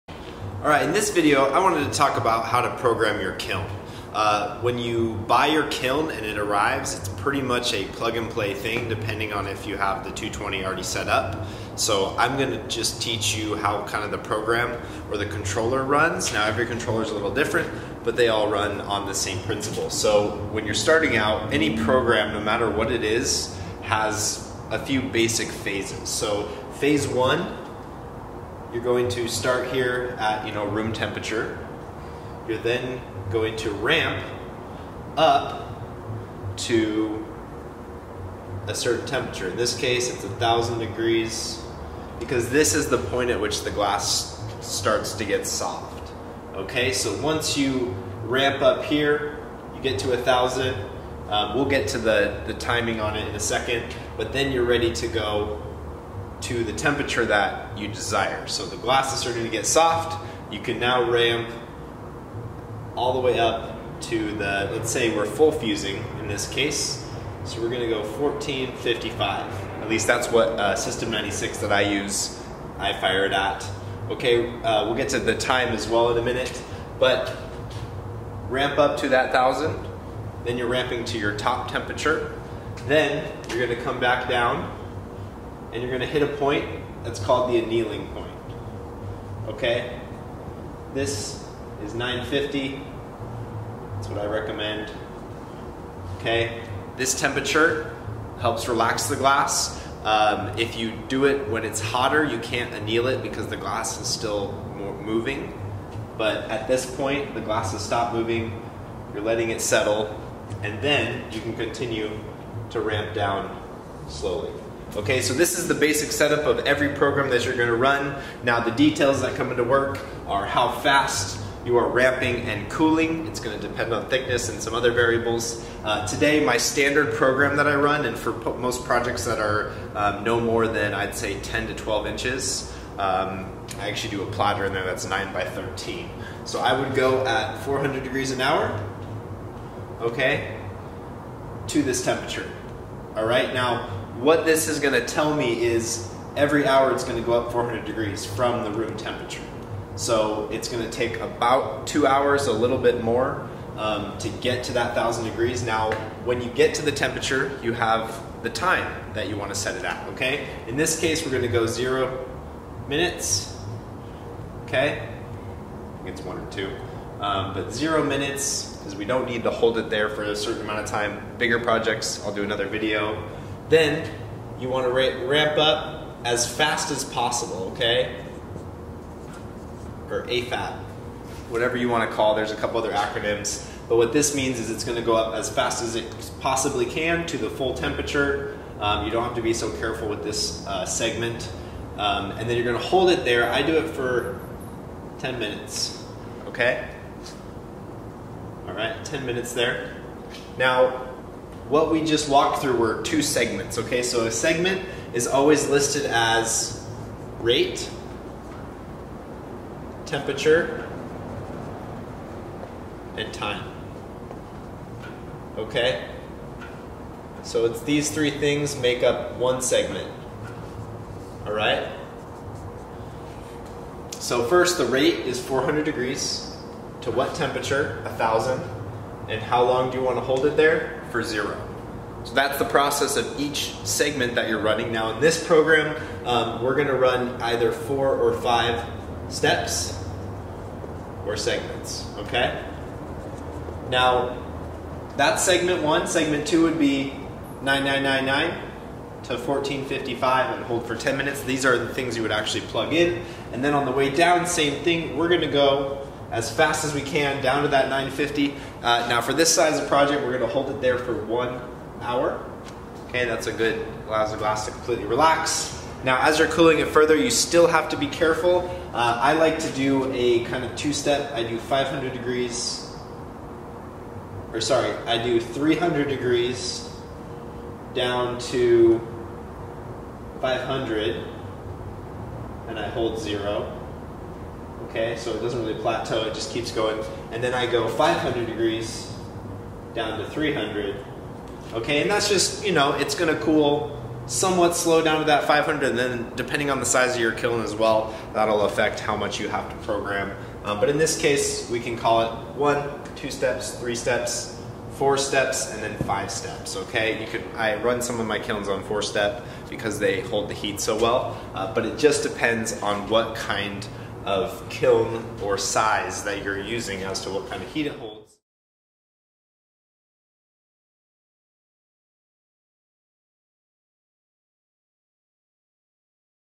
How a Glass Kiln Operates
Wondering how your fused glass kiln runs? Here is a quick guide for basic Glass Firings for system 96 glass. Please leave any questions in the comments below!